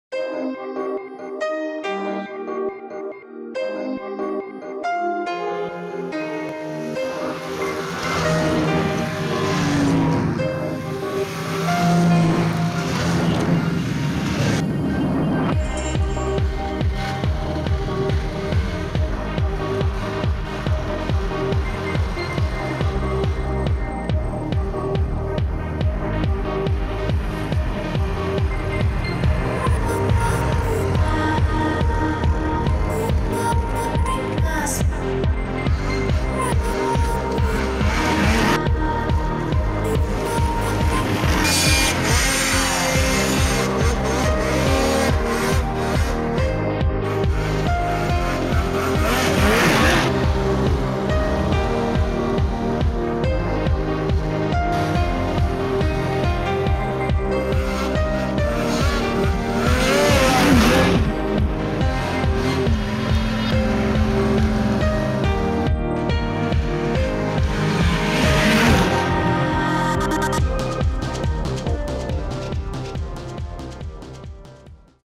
Koneviestin moottorikelkkatesti
Polaris Titan- ja Yamaha Viking Professional EPS -moottorikelkoille on yhteistä niiden tehokas moottori ja erillisjousitettu alusta. Vertailuryhmäksi koottiin muitakin samaan kategoriaan kuuluvia kelkkoja. Lue vertailusta